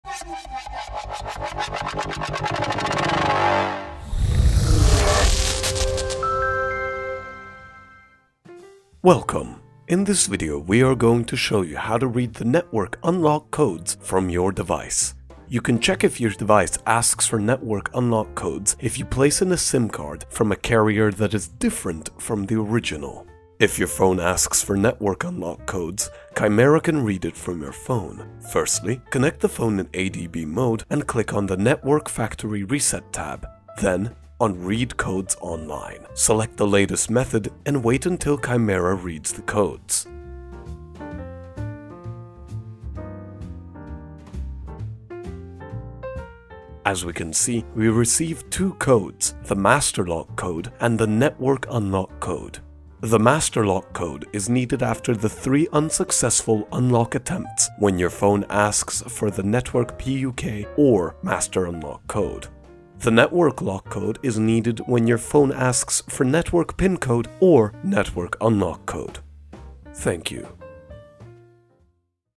Samsung, Android 13, Read Codes Online with Chimera
In this video, we are going to show you the Read Codes Online procedure for Samsung devices. Read Codes Online will read the network unlock codes from your device, by entering these codes you are able to unlock the device from a locked carrier. This method works on the latest binary versions as well as on Android 13.

Supported models:
- SM-G980F (Samsung Galaxy S20)
- SM-G985F (Samsung Galaxy S20 Plus)
- SM-G981B (Samsung Galaxy S20 5G)
- SM-G986B (Samsung Galaxy S20 Plus 5G)
- SM-G988B (Samsung Galaxy S20 Ultra 5G)
- SM-N980F (Samsung Galaxy Note 20)
- SM-N985F (Samsung Galaxy Note 20 Ultra)
- SM-N981B (Samsung Galaxy Note 20 5G)
- SM-N986B (Samsung Galaxy Note 20 Ultra 5G)
- SM-G780F (Samsung Galaxy S20 FE)